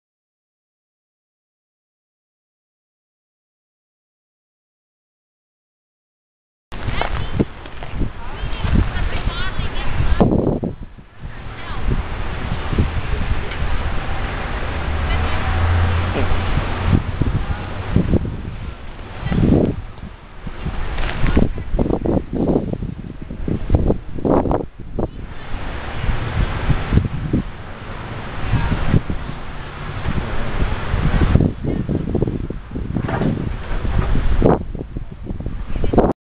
Back On Its Feet
Traffic accident on the corner of Rio Vista Dr. and Hwy. 95, Bullhead, AZ. Dodge pickup wound up on its side, crews are righting the crippled truck.